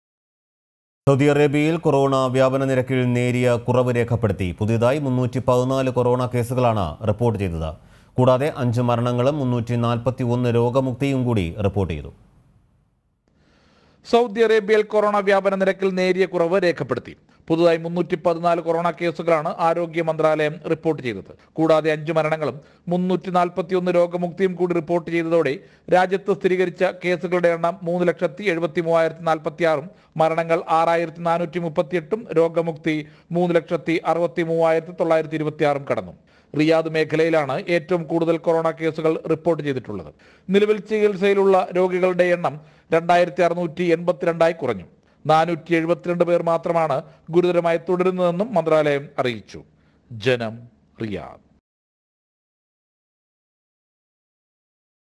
സൗദി അറേബ്യയിൽ കൊറോണ വ്യാപന നിരക്കിൽ നേരിയ കുറവ് രേഖപ്പെടുത്തി
NEWS ANCHOR : G KISHORE